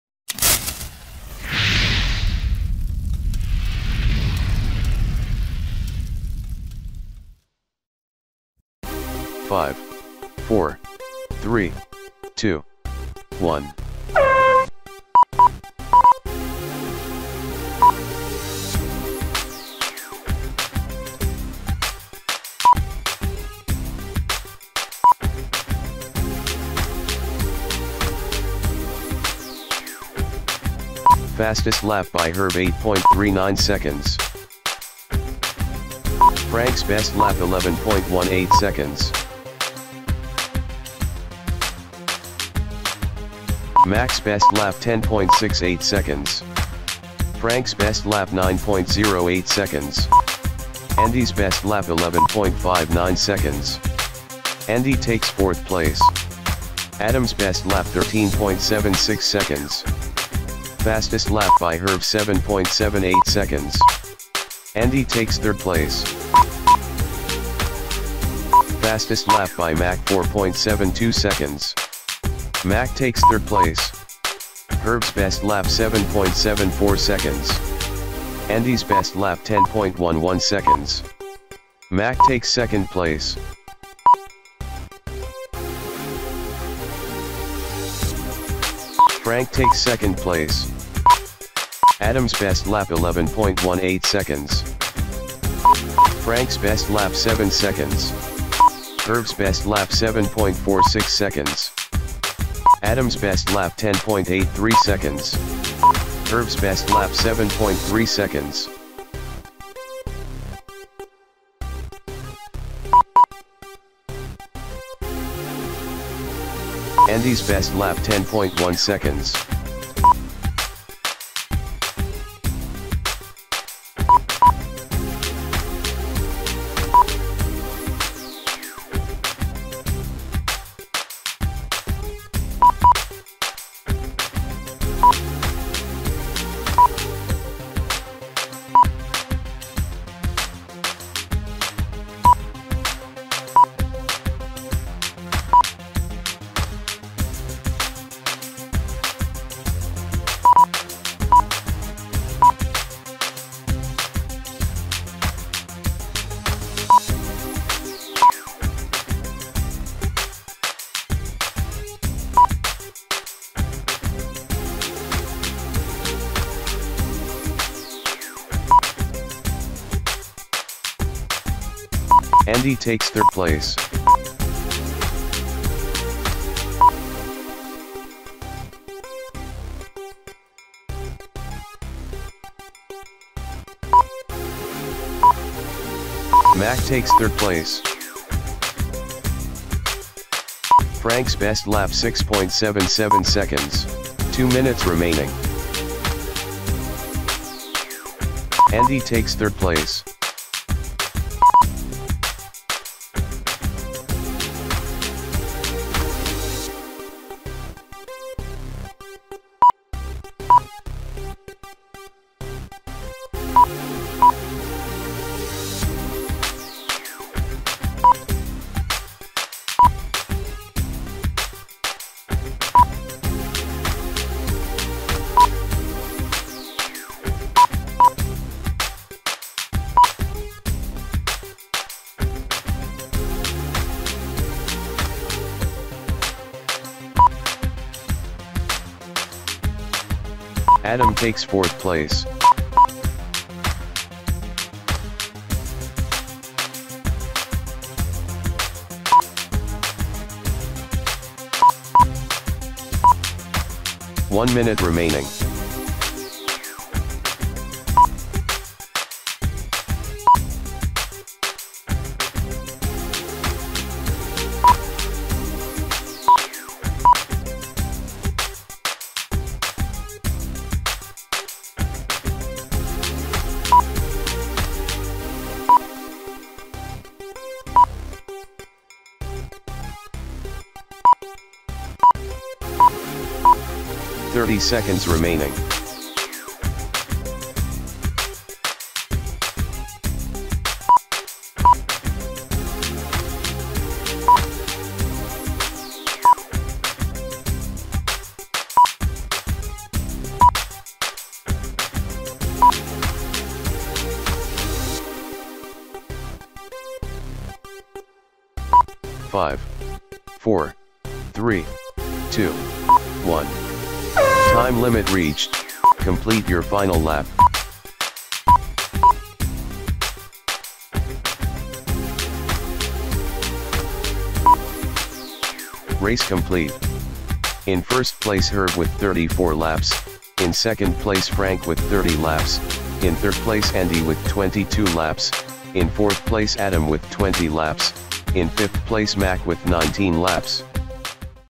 118 Winter Series II R5 H2
Race Day - Race 5 - Heat 2

Heat Winner - Hrv P
Fastest Lap - Frank M - 6.77